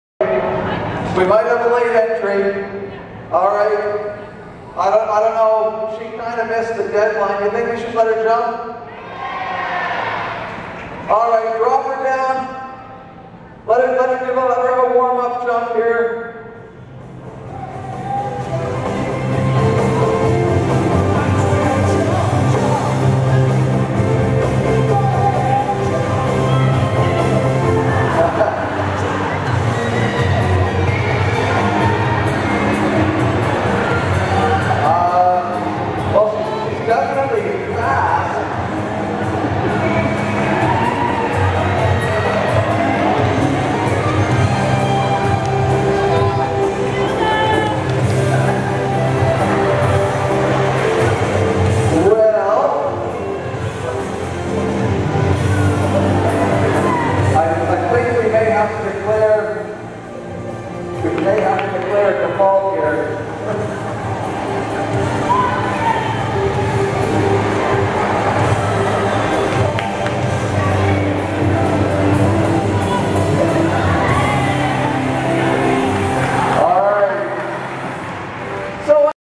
Gilda gets the zoomies at Old Home Week
Gilda the Ibizan Hound gets the zoomies instead of doing the high jump.